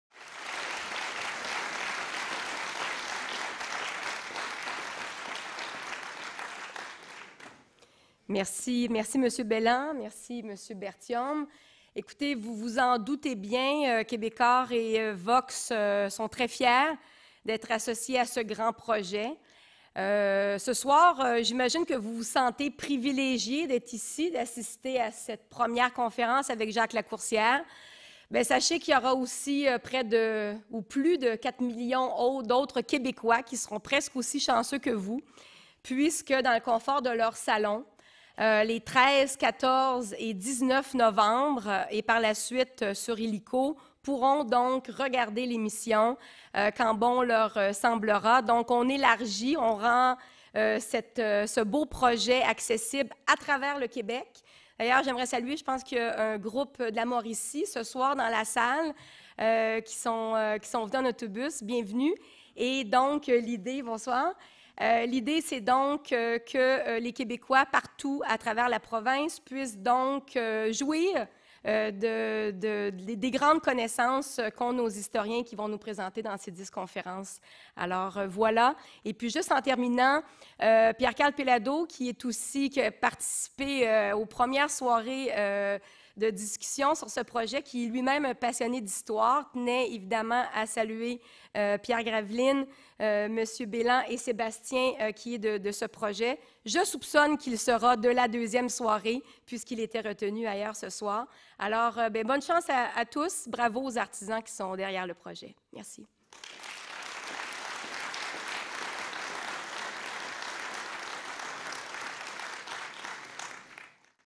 Allocution de Isabelle Dessureault, première conférence, 13 octobre 2011
Allocution d'Isabelle Dessureault, présidente de VOX, lors de la première conférence de la série «Dix journées qui ont fait le Québec», le 13 octobre 2011.